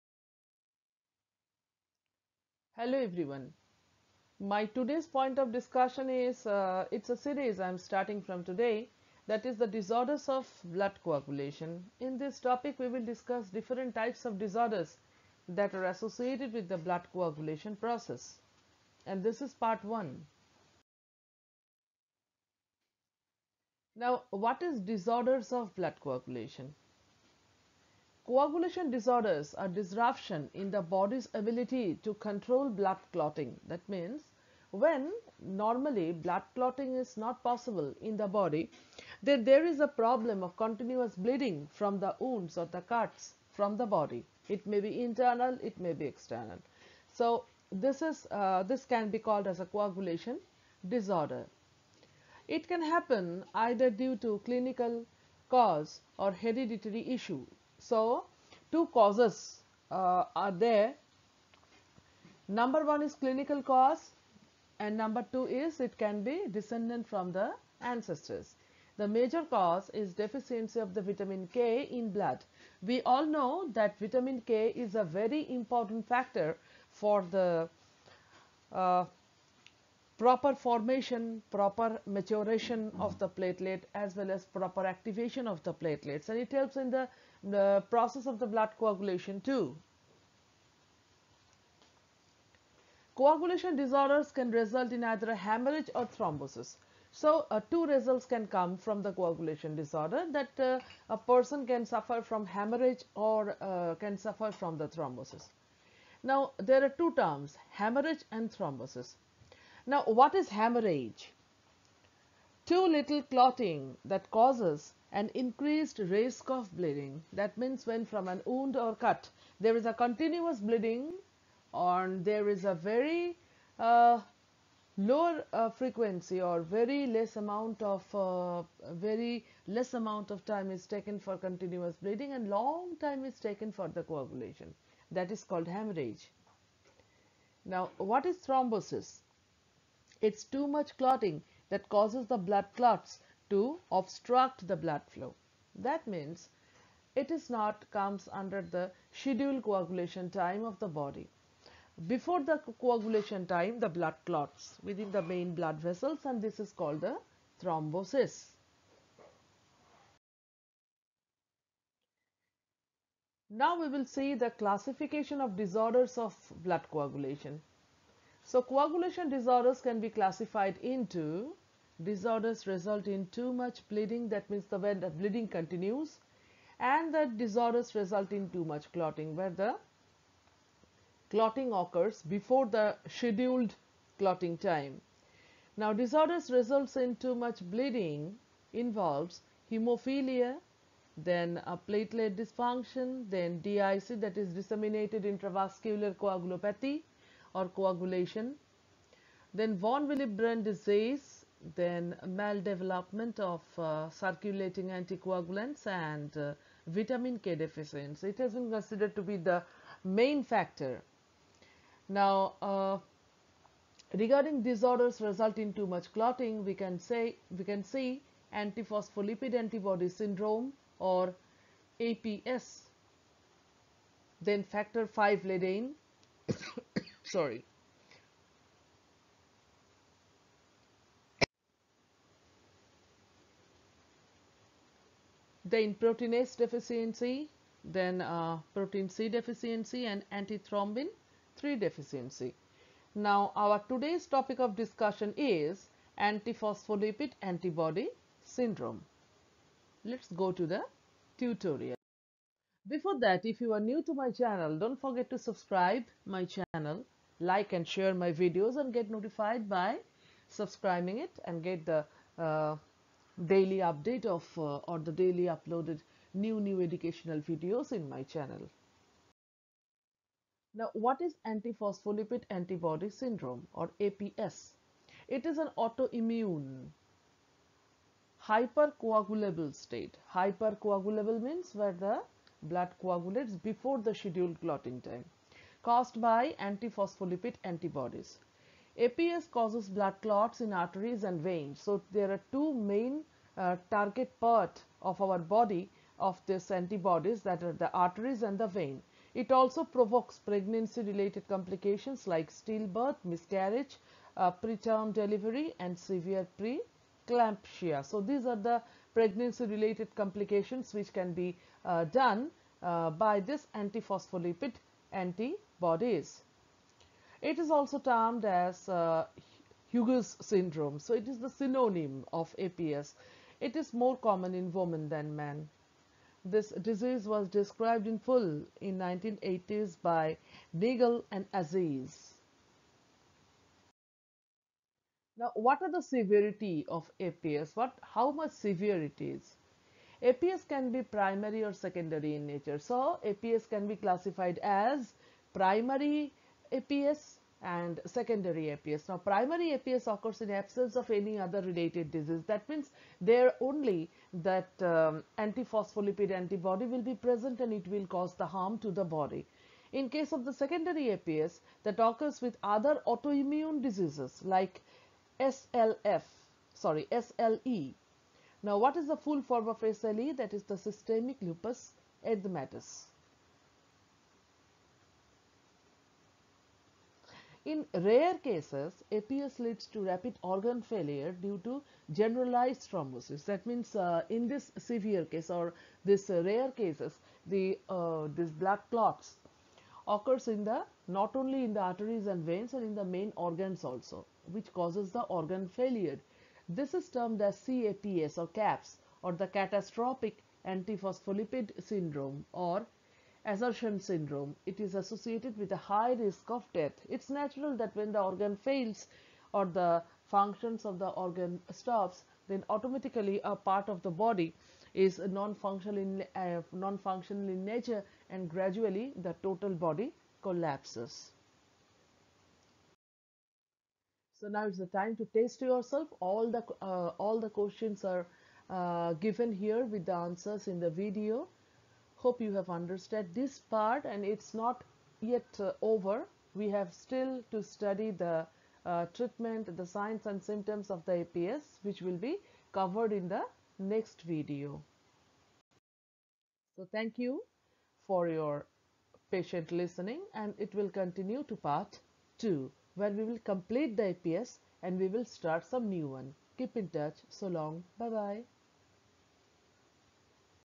Disorders of Blood Coagulation (part 1)
This video series describes regarding different types of bleeding disorders in detail. It is very important for medical and paramedical students.